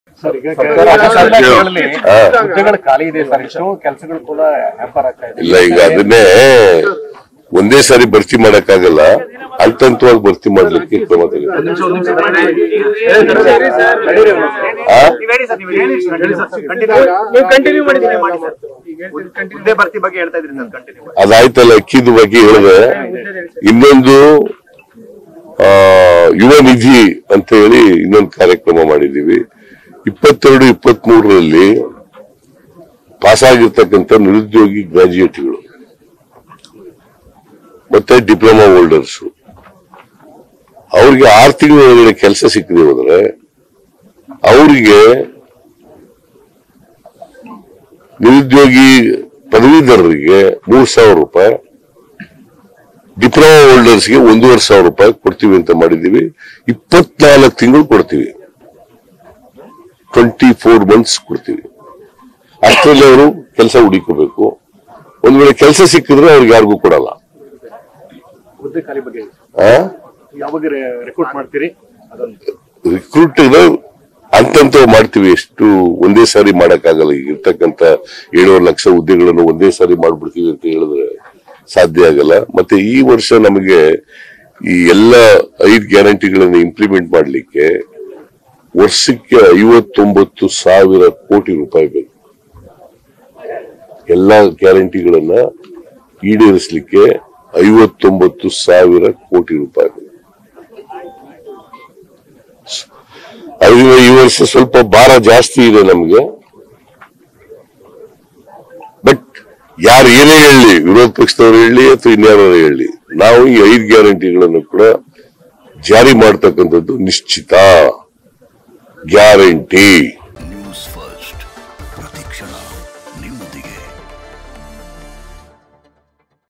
CM Siddaramaiah : ಎಲ್ಲ ಗ್ಯಾರಂಟಿ ಈಡೇರಿಸಕ್ಕೆ ಎಷ್ಟು ಕೋಟಿ ಬೇಕು ಗೊತ್ತಾ? | Congress Guarantees | Newsfirst
CM Siddaramaiah : ಎಲ್ಲ ಗ್ಯಾರಂಟಿ ಈಡೇರಿಸಕ್ಕೆ ಎಷ್ಟು ಕೋಟಿ ಬೇಕು ಗೊತ್ತಾ? | Congress Guarantees | @newsfirstkannada